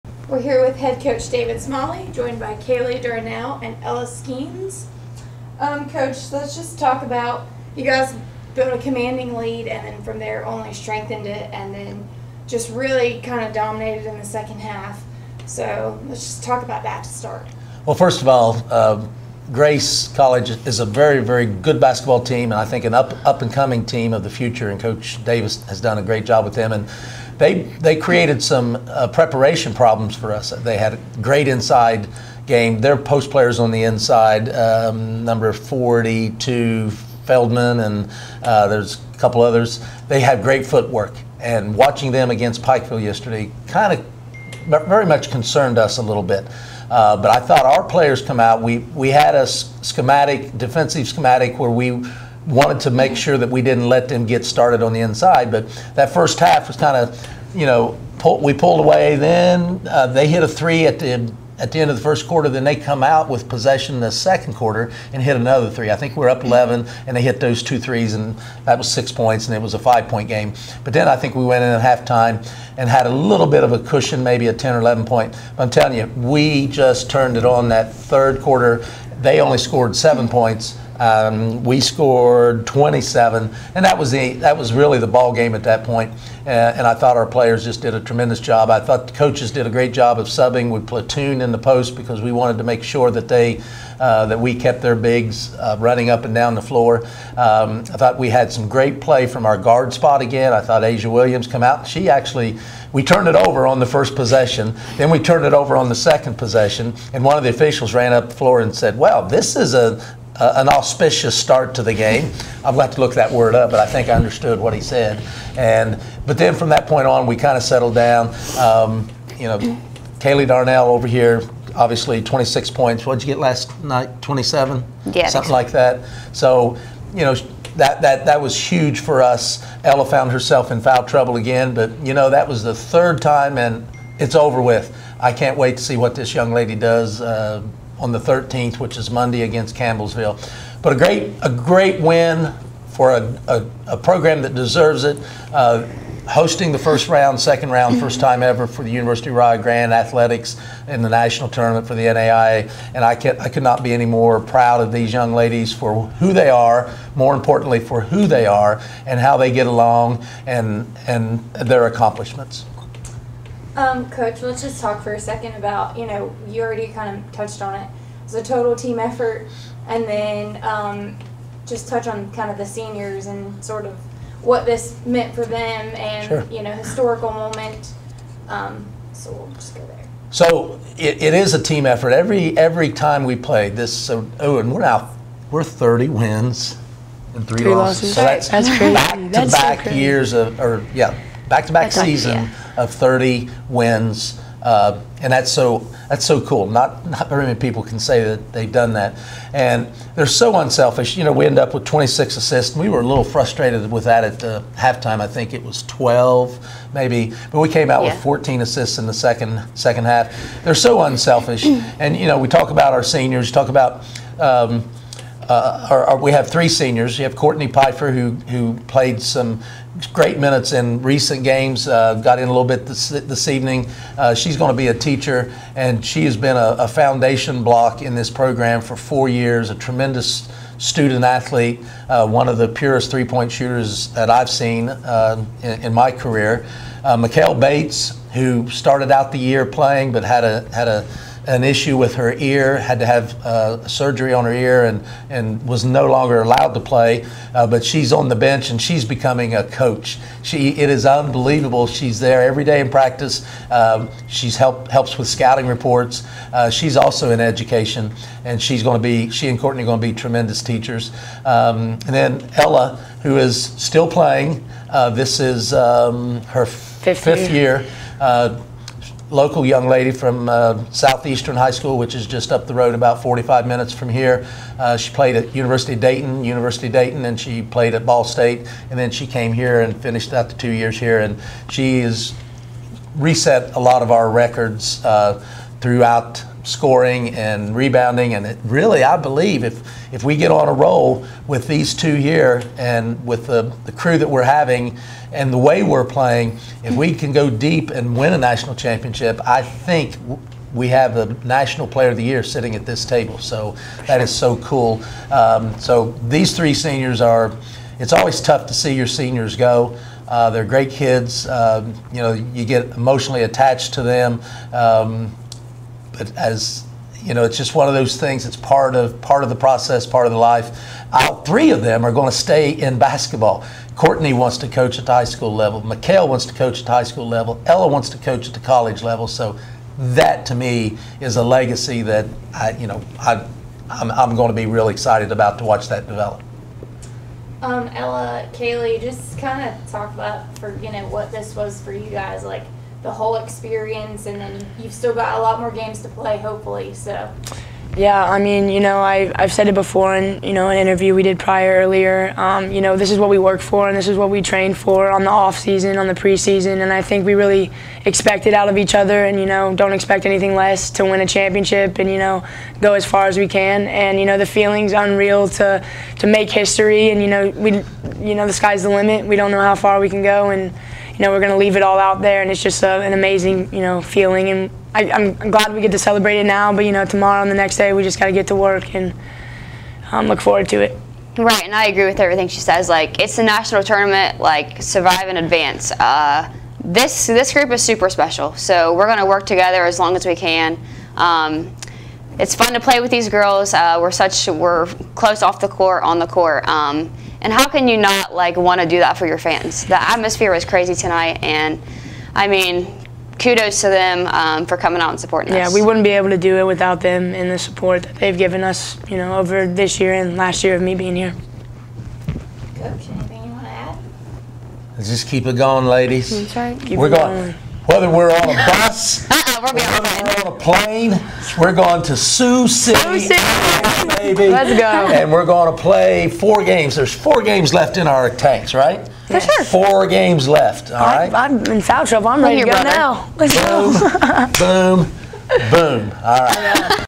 University of Rio Grande Post Game Championship
Women's basketball post-game interview after winning the first and second round championship.